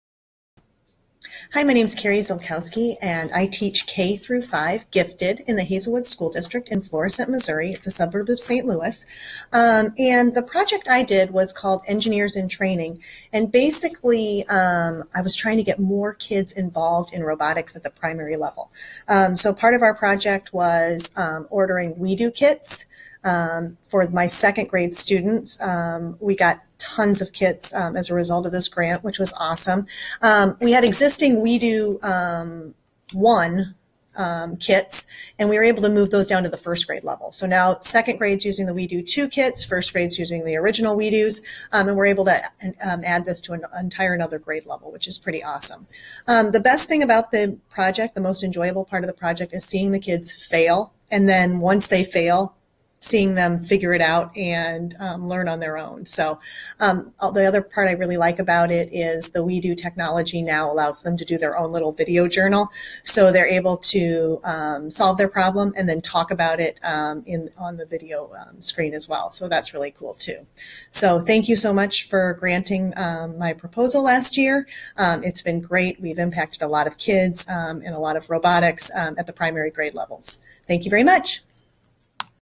Carrie Ziolkowski
NEA Foundation grantee Carrie Ziolkowski discusses how her grant helped her engage her students in STEM learning using LEGO WeDo 2.0 kits.